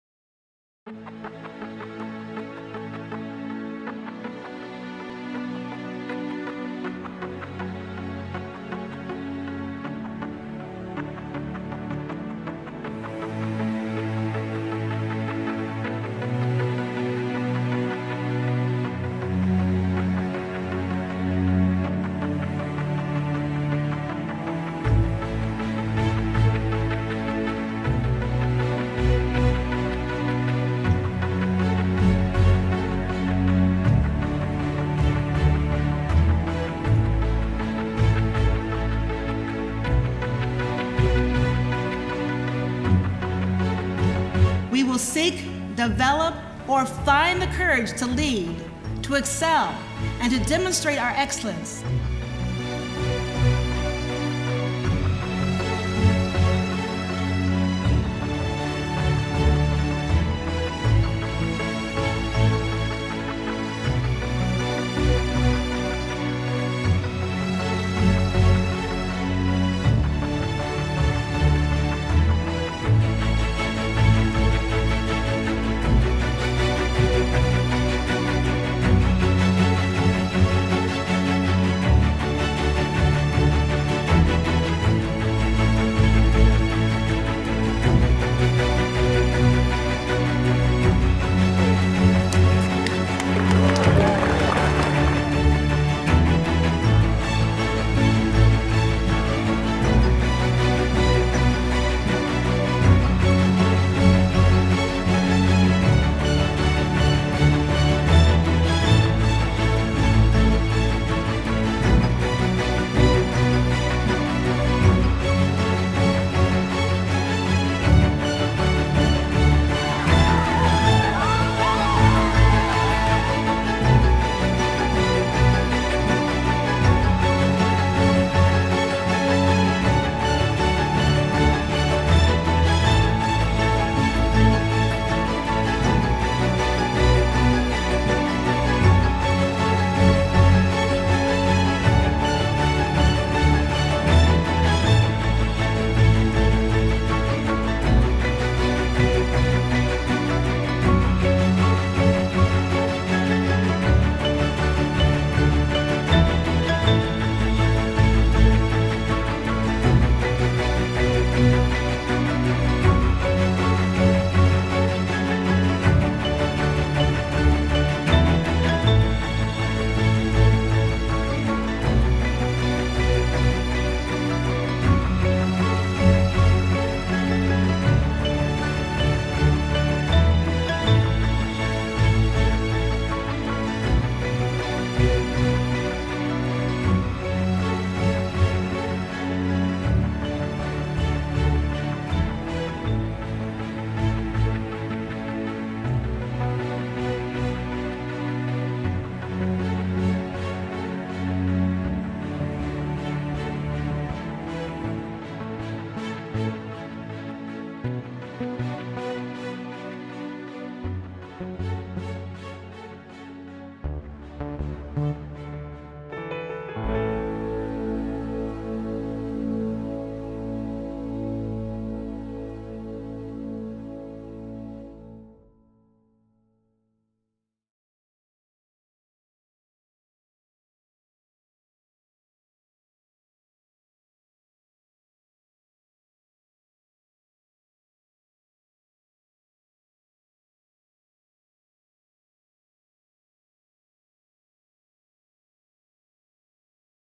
Buffalo State 2014-2015 Highlights
During President Katherine Conway-Turner's Fall Forum on September 29, 2015, she presented highlights from the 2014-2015 academic year and set the direction forward for the upcoming strategic planning process.